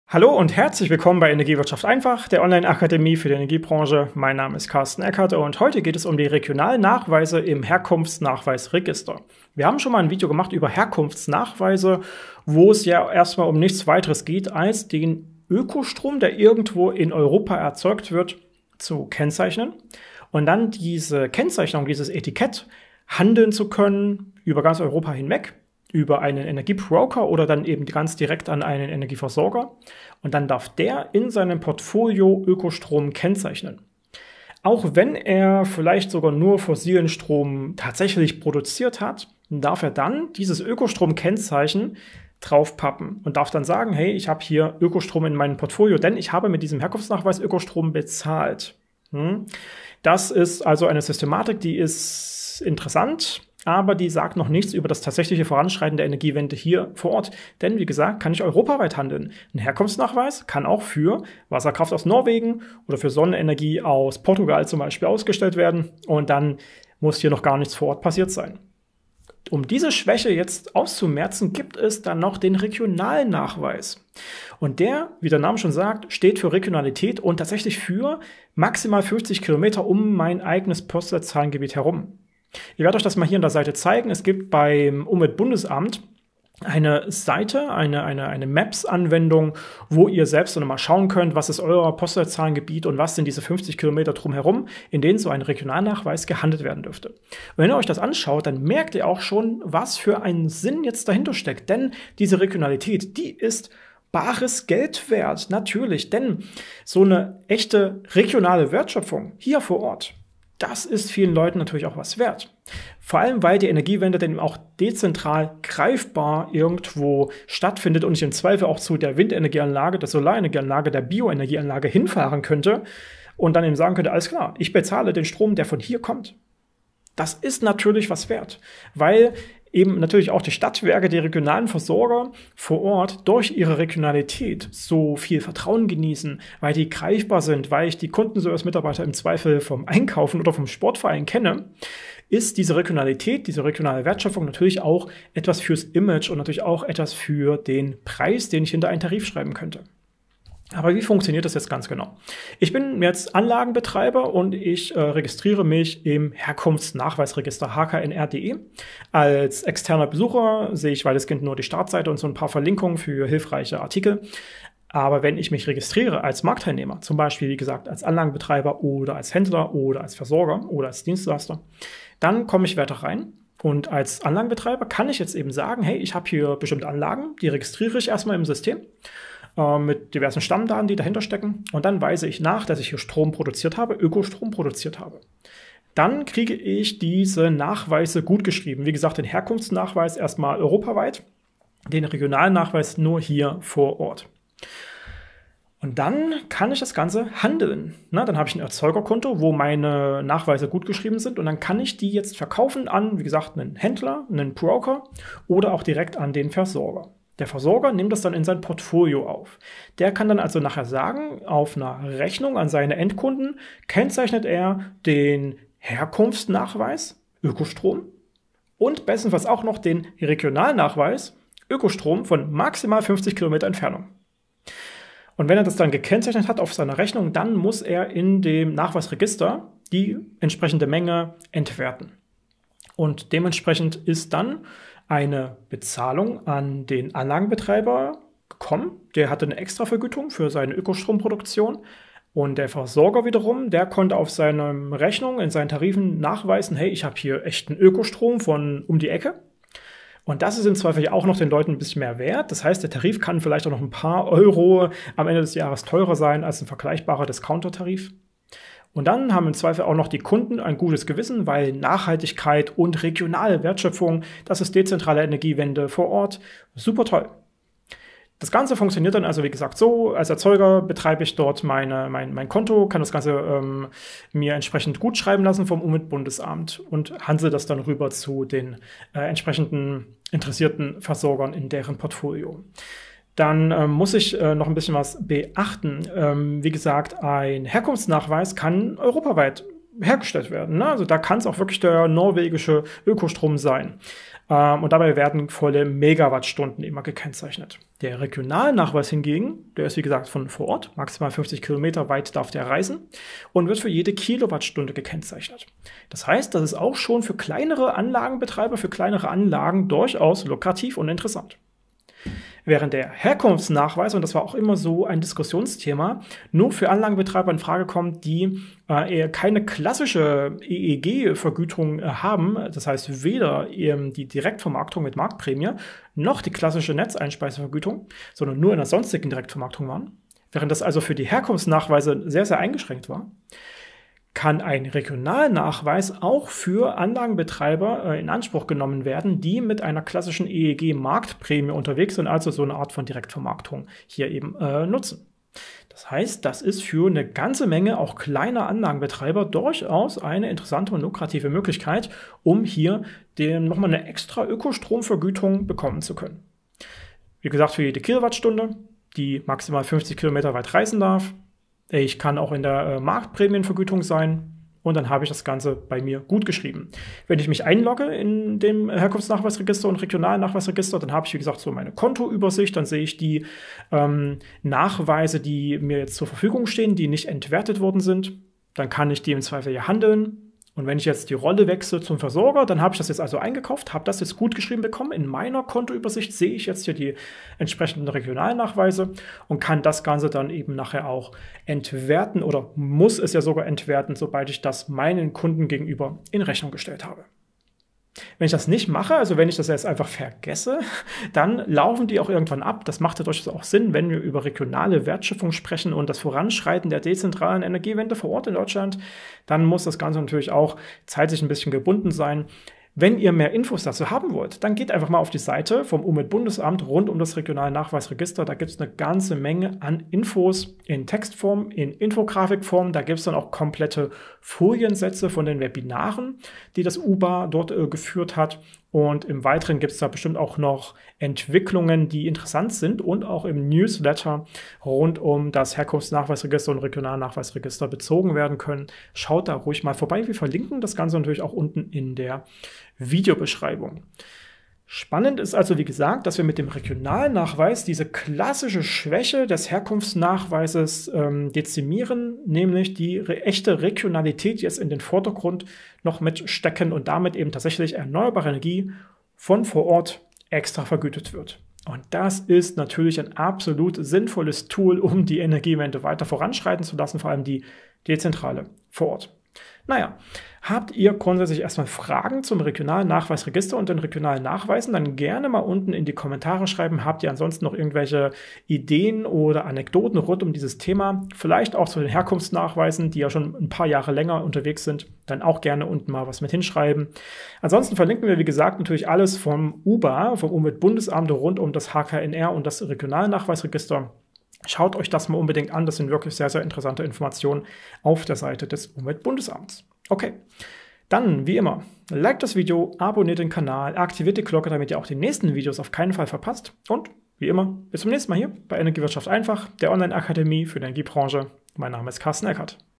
Regionalnachweise für Erneuerbaren Strom
Regionalnachweise erlauben die Kennzeichnung Erneuerbaren Stroms aus unmittelbar benachbarter Produktion. So wie die ursprünglichen Herkunftsnachweise werden sie im HKNR gebildet, gehandelt und nach Kennzeichnung entwertet.